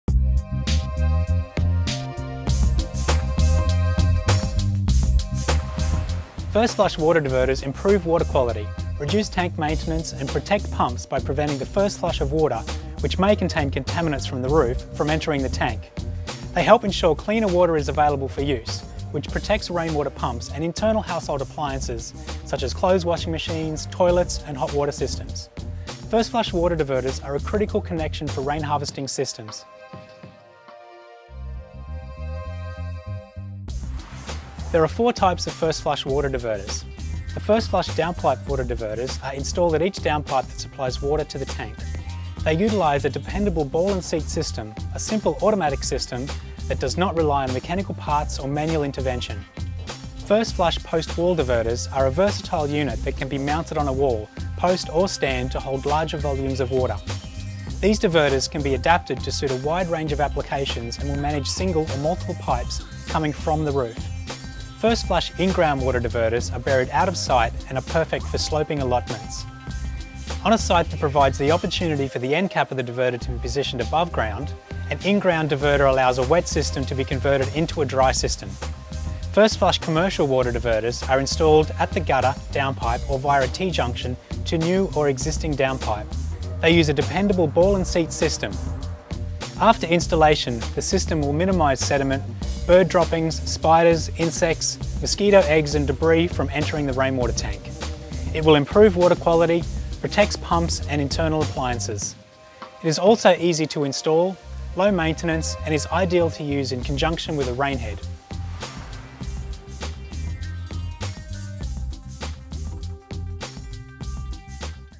First Flush Water Diverters
Rain Harvesting - First Flush Water Diverters improve water quality, reduce tank maintenance and protect pumps by preventing the first flush of water, which may contain contaminants from the roof, from entering the tank.

Why use First Flush Diverters?